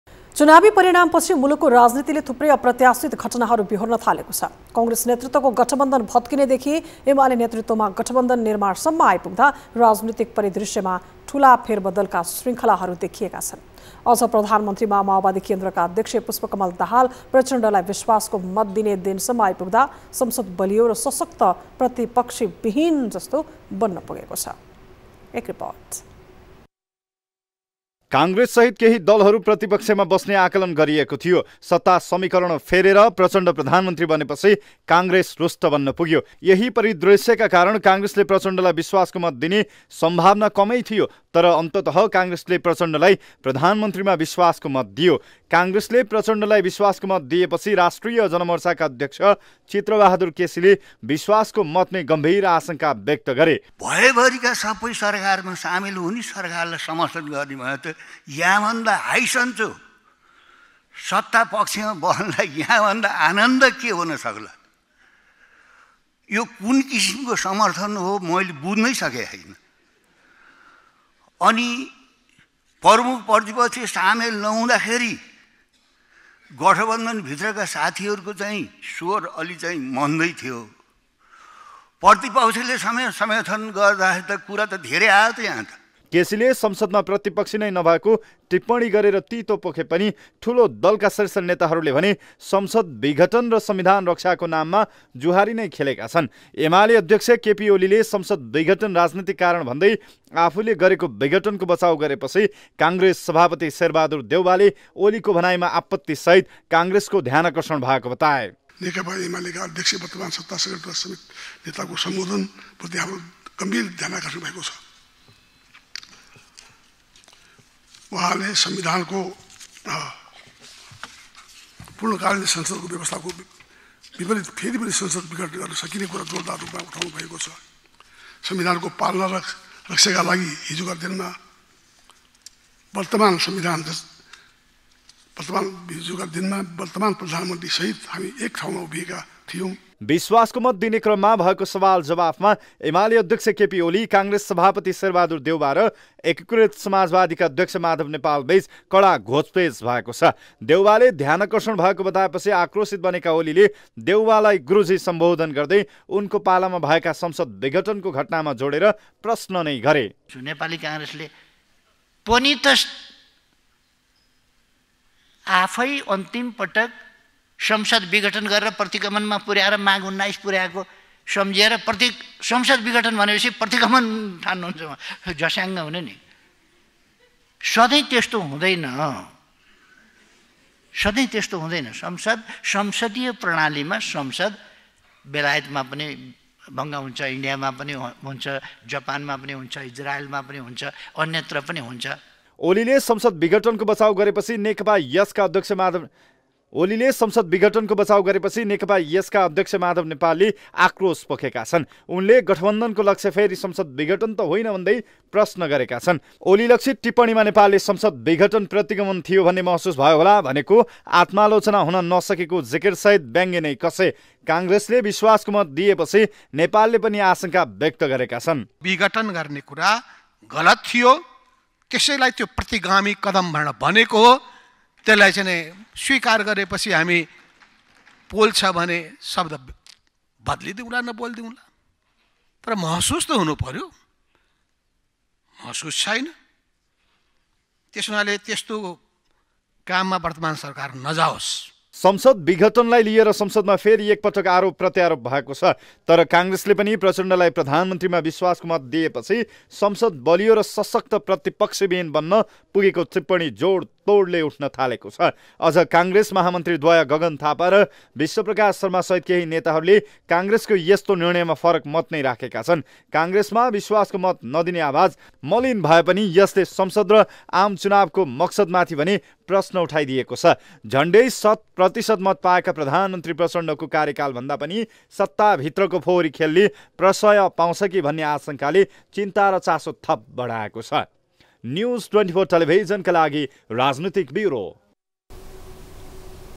संसदमा सिंगुरीः संसद विघटन गर्ने कि नगर्ने ? - NEWS24 TV
चुनावी परिणामपछि मुलुकको राजनीतिले थुप्रै अप्रत्यासित घटनाहरु ब्यहोर्न थालेको छ । कांग्रेस नेतृत्वको गठबन्धन भत्किनेदेखि एमाले नेतृत्वमा गठबन्धन निर्माणसम्म आइपुग्दा राजनीतिक परिदृश्यमा ठूला फेरबदलका श्रृङ्खलाहरु देखिएका छन् । अझ प्रधानमन्त्रीमा माओवादी केन्द्रका अध्यक्ष पुष्पकमल दाहाल ‘प्रचण्ड’लाई विश्वासको मत दिने दिनसम्म आइपुग्दा संसद बलियो र सशक्त प्रतिपक्षीबिहीन जस्तो बन्न पुगेको छ ।